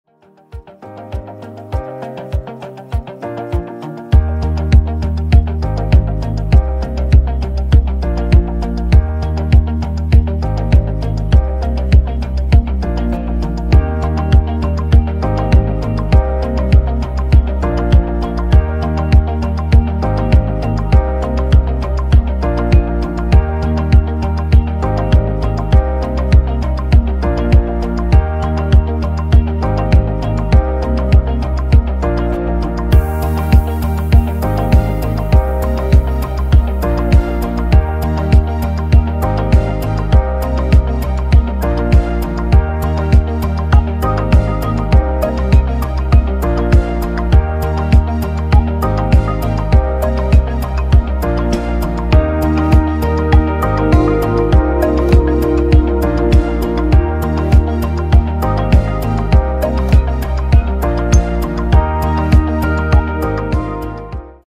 How to copy/clone a DVT-PRO TPMS Sensor with your ATEQ or ATEQ-branded TPMS Tool.
Learn how to copy a DVT-PRO TPMS Sensor (ENGLISH) using a DY-47 TPMS Tool (Powered by ATEQ).